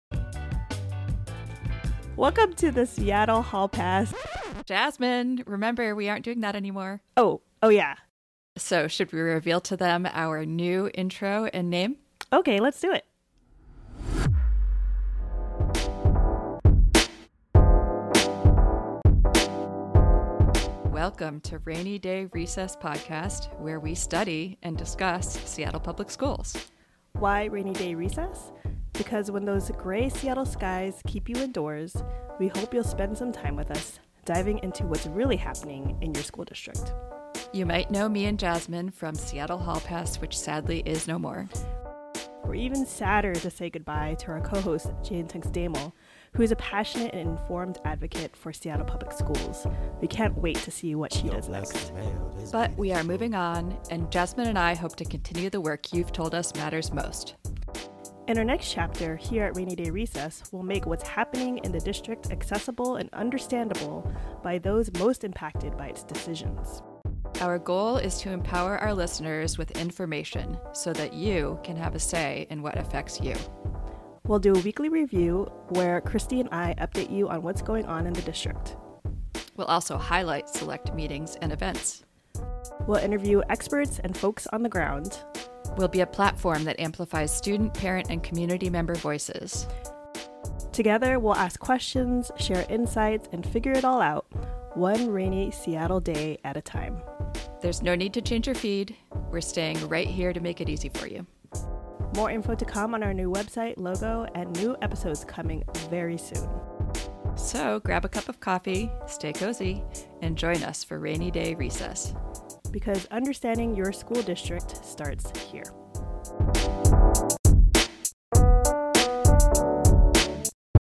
Introducing Rainy Day Recess
In this introductory trailer for Rainy Day Recess, hosts Christie Robertson and Jasmine Pulido introduce their new podcast, which focuses on making Seattle Public Schools' decisions and policies accessible and understandable to the community. As former hosts of Seattle Hall Pass, they reflect on their journey and bid farewell to their co-host Jane Tunks Demel.

They outline their mission to empower listeners with weekly updates, event highlights, expert interviews, and stories from students, parents, and community members. With a focus on collaboration and understanding, the hosts aim to help listeners navigate Seattle's school district "one rainy day at a time."

Rainy Day Recess music by Lester Mayo, logo by Cheryl Jenrow.